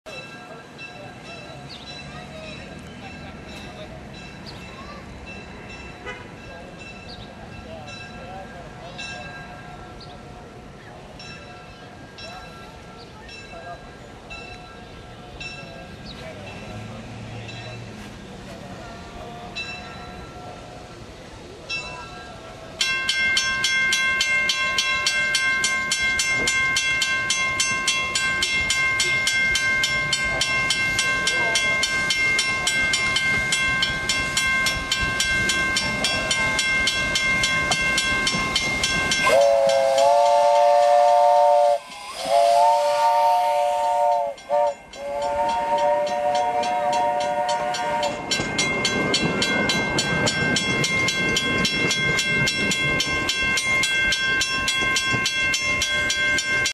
The Poway Midland Railroad Steam Locomotive No. 3
Highly recommended that you watch in 480p. This is a fun little 1/2 mile long train ride in Poway, CA.

One well kept secret in the greater San Diego area is the Poway Midland Railroad, located as the name would imply in the city of Poway. I won't go into too many details, suffice it to say, it is a small, but cool little operation. They have a few interesting pieces of equipment that they run on the weekends - a LARY trolley (battery powered), an old speeder car and short train, and probably the most interesting, an old 1907 steam locomotive, completely refurbished. However, the days of the steam locomotive may be numbered. A crack was recently found and patched in the 70 year old boiler, but the next time it breaks, the railroad will have to purchase a new boiler - a very expensive ordeal. I don't know how far into the future that will be, but I highly recommend that if you have the chance and would like to see the steamer, you do so not too long from now. It's definitely worth the visit.